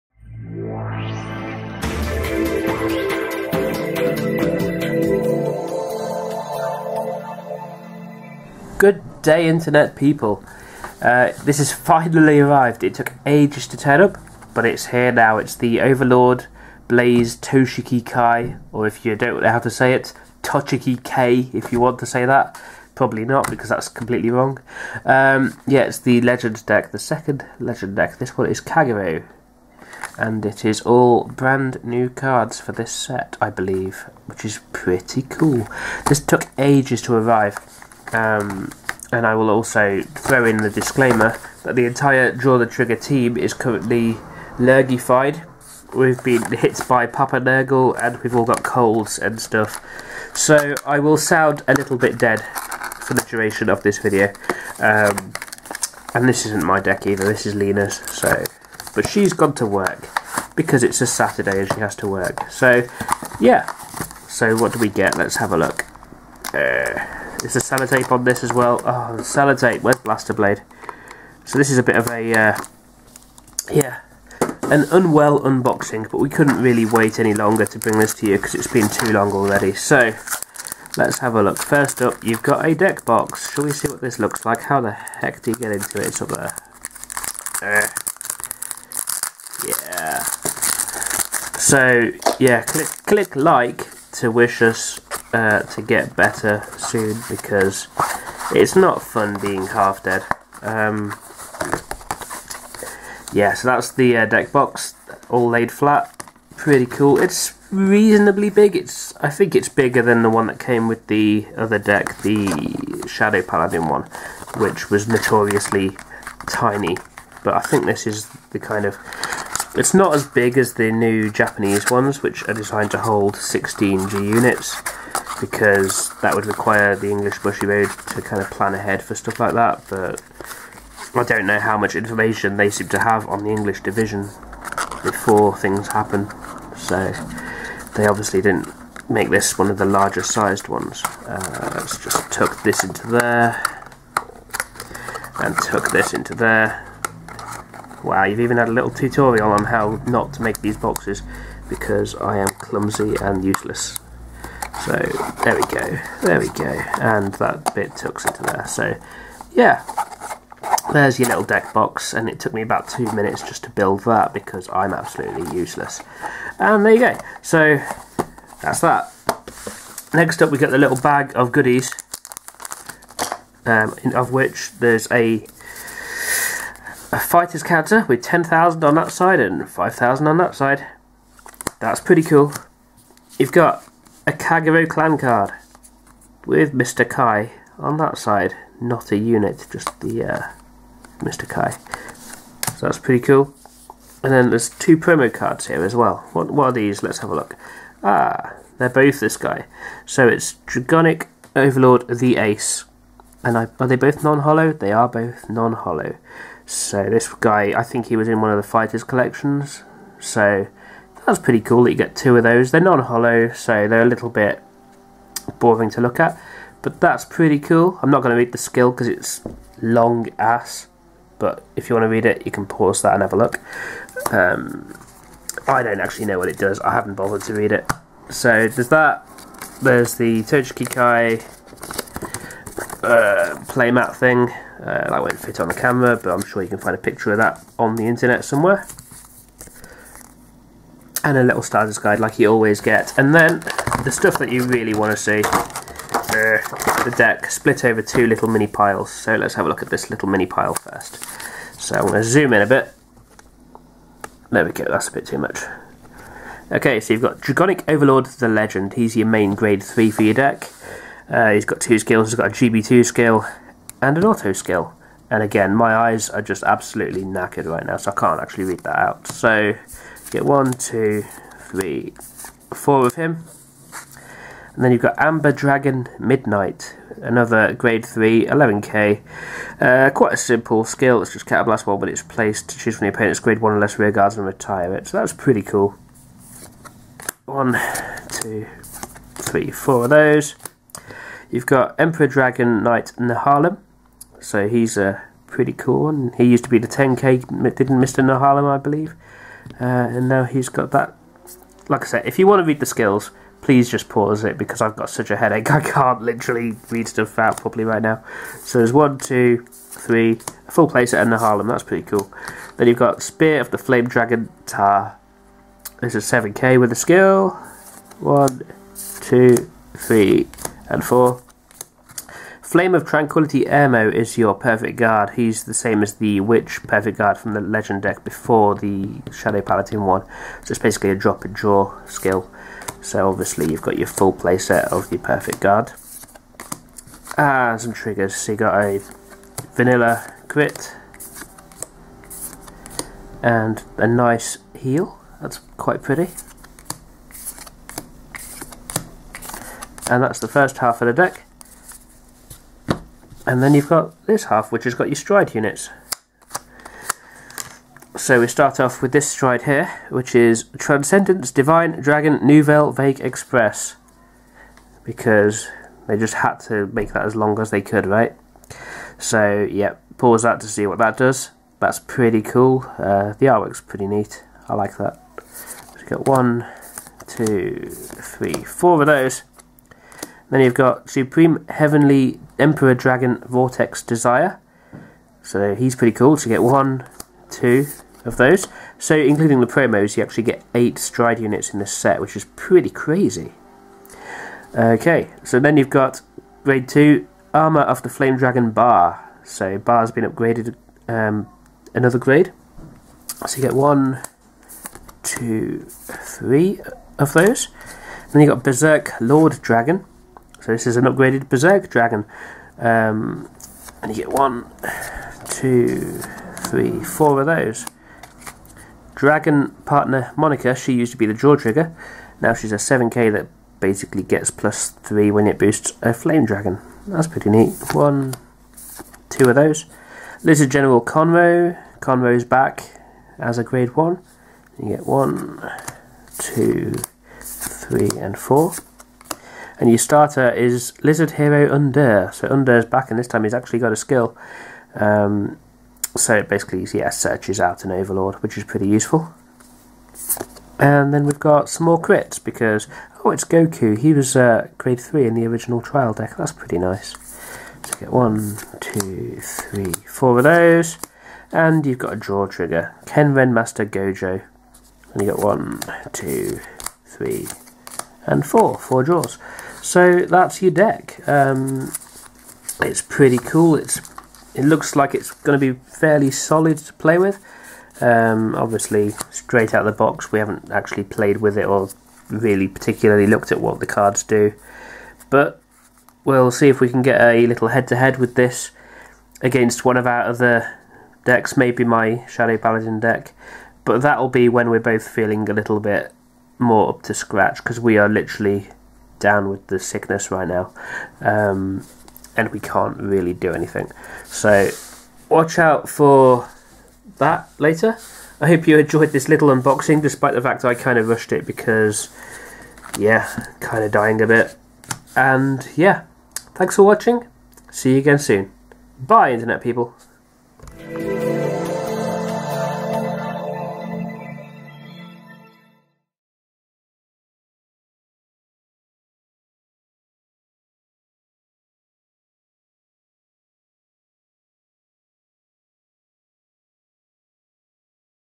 Unboxing Kai Toshiki Kagero Legend Deck - Cardfight!! Vanguard
...In which Phil opens up Kagero's latest support.

Like our playmats? for all the info you need to get your own playmat!